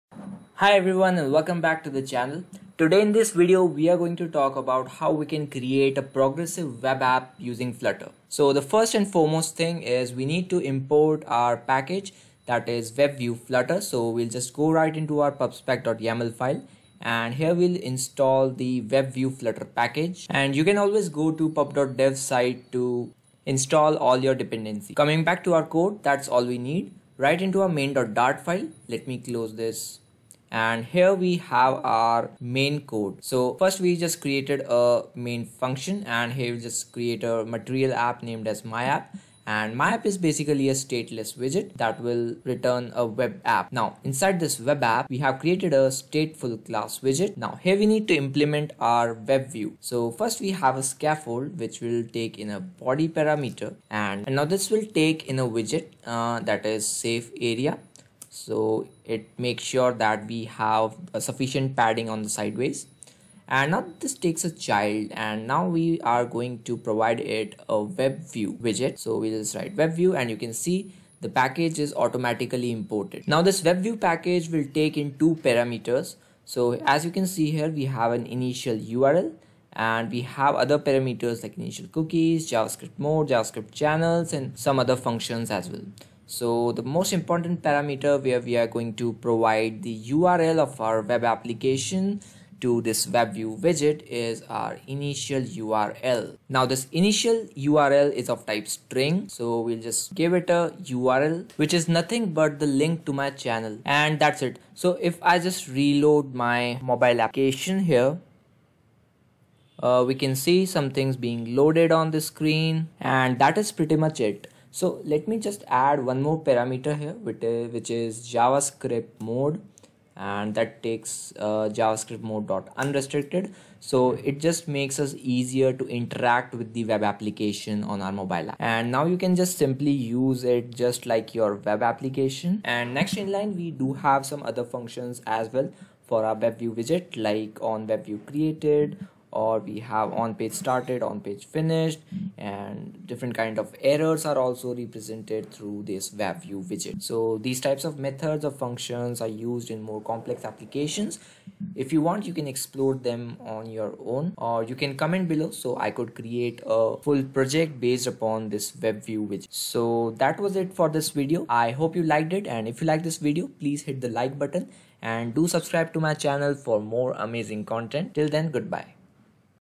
Web view in Flutter | Flutter WebView tutorial
In this tutorial, we'll dive into the world of Flutter and WebView to show you how to create web view in mobile app with ease.  Web view Apps are a fantastic way to combine the best of the web and native apps. We'll guide you through the process step by step, from setting up your Flutter project to optimizing your web view app for a seamless user experience.